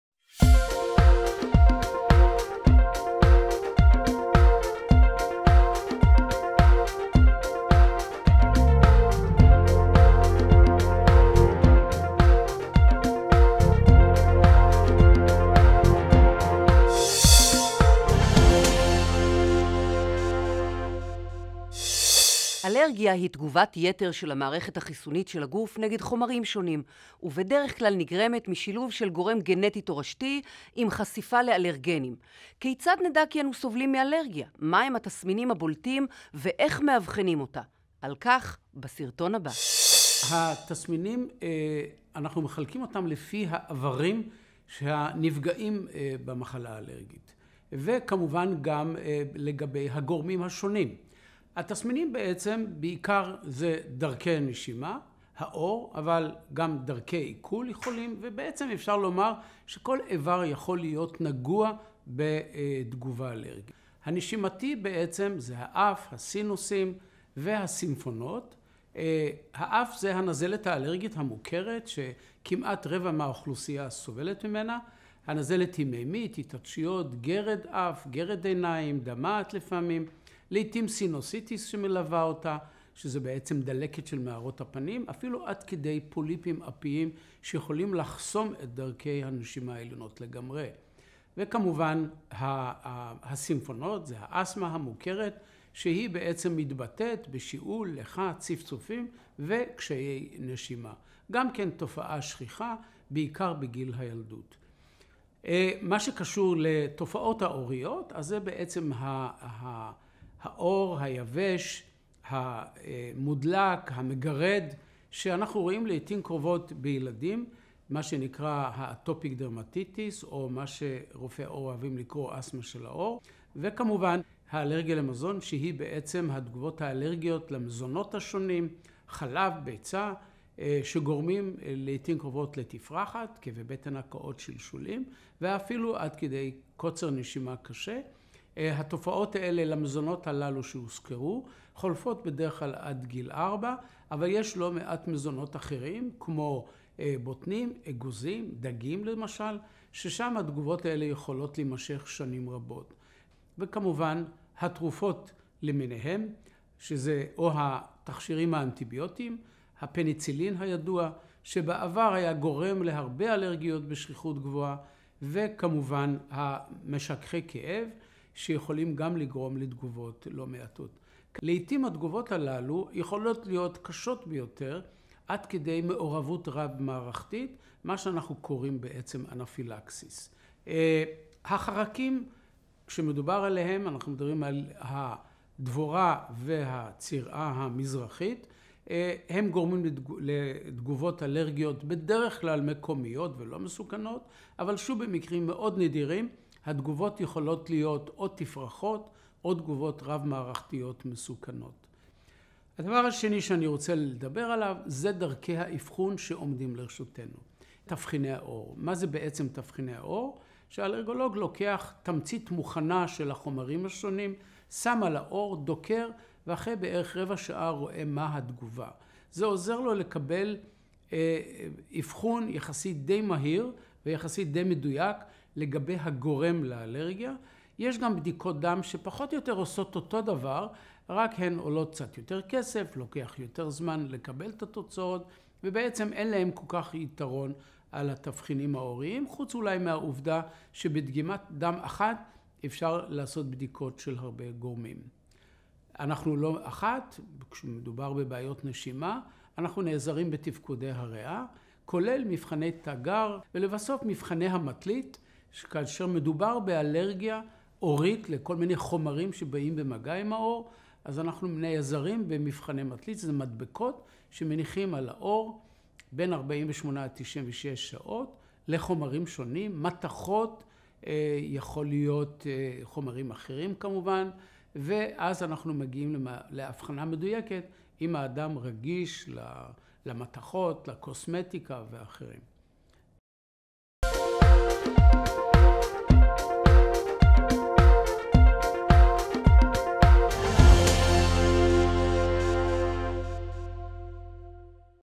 אלרגיה: תסמינים ואבחון
אנשים שונים מפתחים תגובות אלרגיות שונות. התגובות שונות באופיין, למשל, גודש באף ופריחה בעור, ובעוצמתן, למשל, פריחה קלה ומינימלית לעומת פריחה קשה ומפושטת, גודש באף לעומת תגובה סוערת ביותר המכונה אנפילקסיס שעלולה לסכן חיים. פרופ' שמואל קיויתי, מנהל היחידה לאלרגיה ואימונולוגיה, במרכז הרפואי איכילוב,  מסביר מהם התסמינים העיקריים של אלרגיה וכיצד מאבחנים אותה.